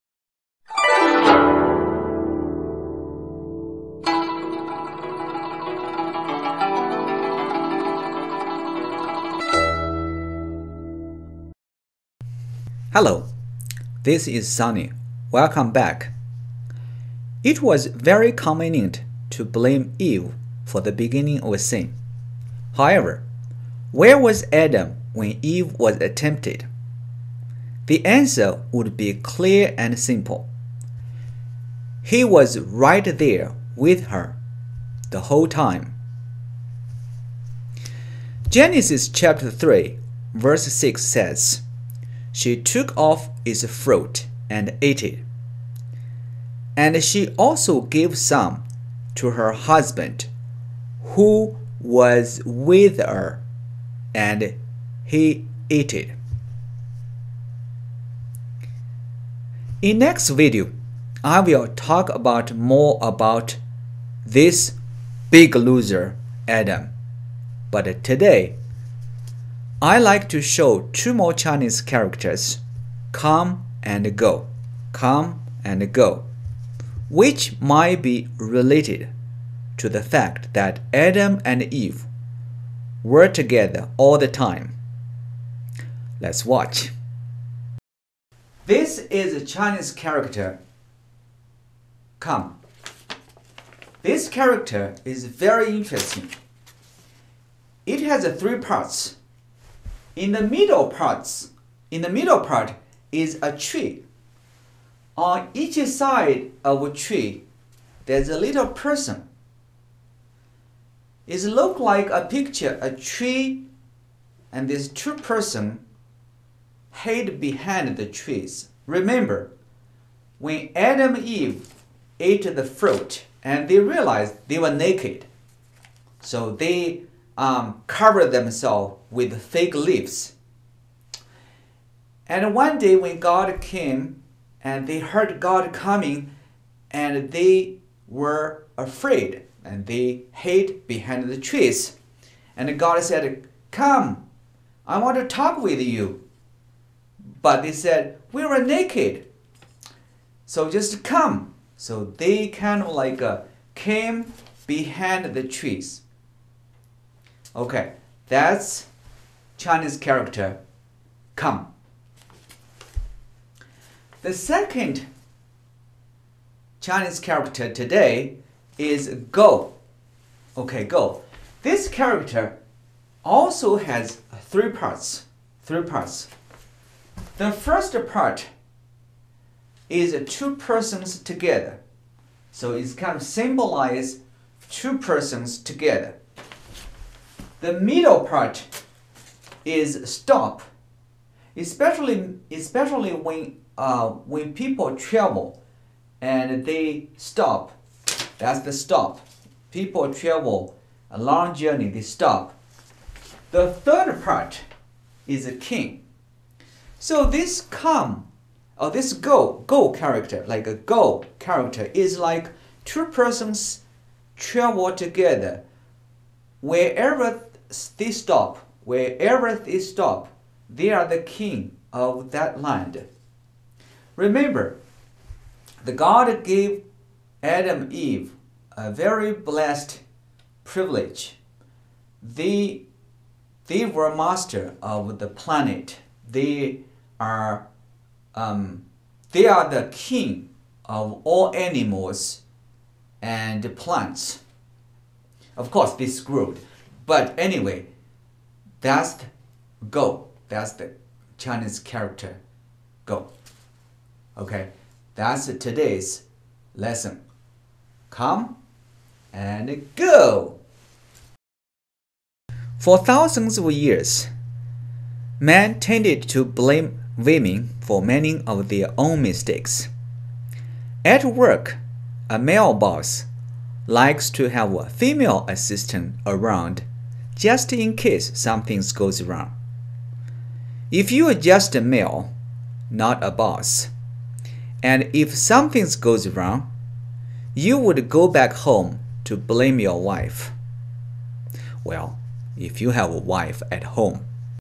She was not alone(来，往): Story behind Chinese characters (12/25)
Please check 25 video series: Story Behind Chinese Characters

All credits should go to C.H. Kang and Nethel Nelson who coauthored the book " The discovery of Genesis : how the truth of Genesis were hidden in Chinese characters"

Earlier in the chapter 3 in Genesis, the story seems to suggest that the only two characters present are Eve and serpent, but verse Chapter 3 verse 6 tells us that Adam was with her. Adam was with Eve when the serpent deceived her.
Today's two Chinese characters reflected the fact that they were together all the time, either "come" or "go".  For the beginning of sin, she was not alone.

This is the part 12 out of 20 Chinese character series: Story Behind Chinese characters.